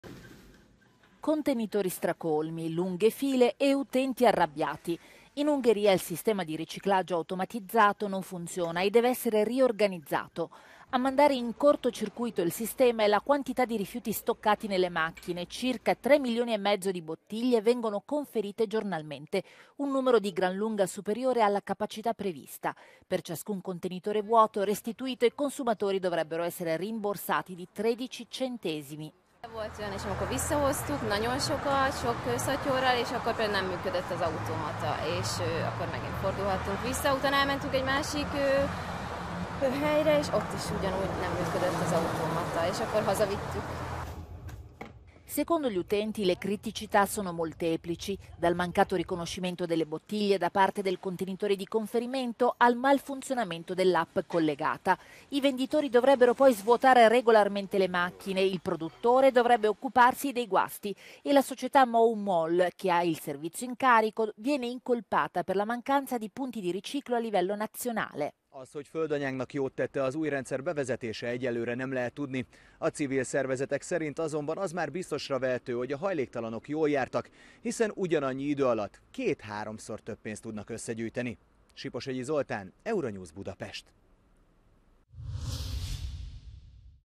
Ungheria: troppi problemi, il sistema di riciclo dei rifiuti non decolla
Circa 3 milioni e mezzo di bottiglie vengono conferite giornalmente, un numero di gran lunga superiore alla capacità prevista dal sistema automatizzato